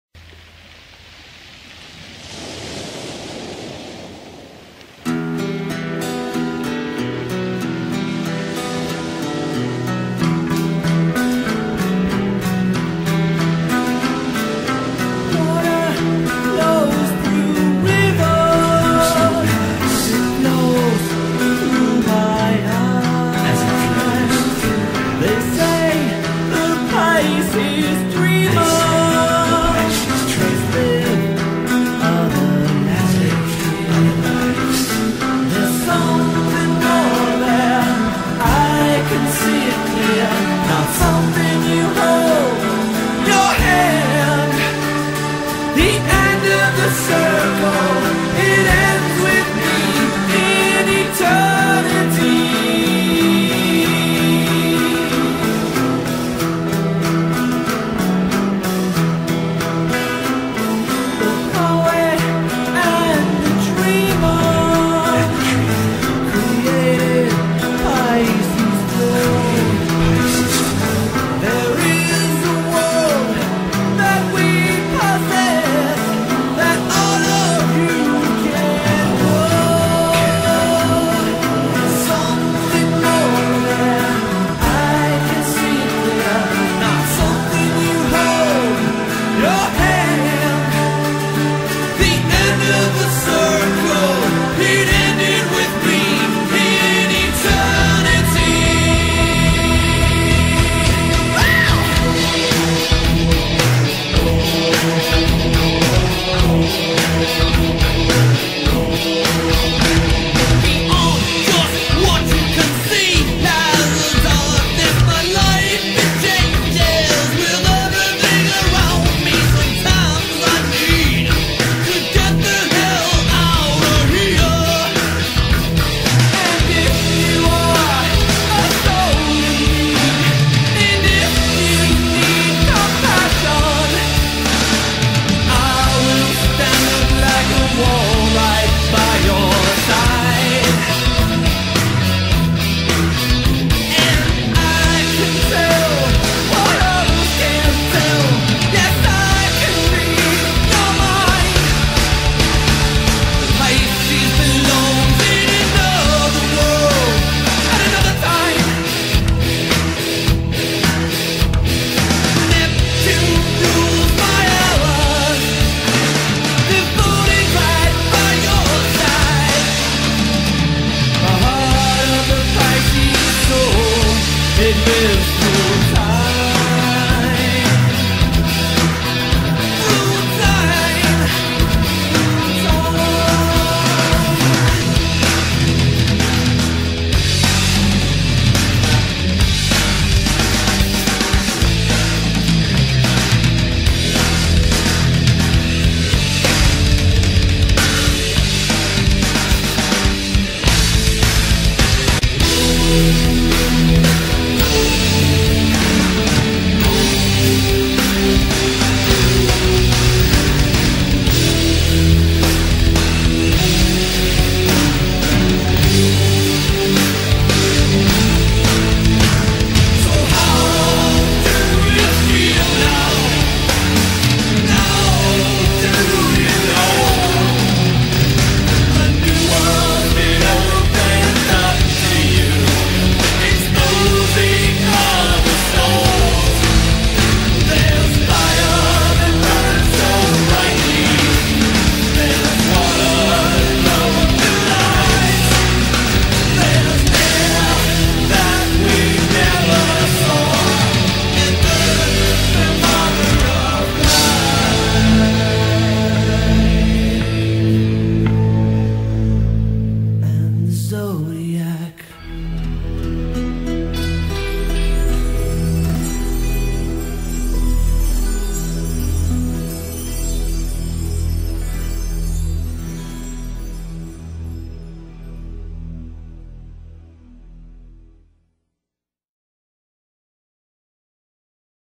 Famous Pisces 3
I do believe this is my final pisces video. After TONS of research, I think I finally got most of them.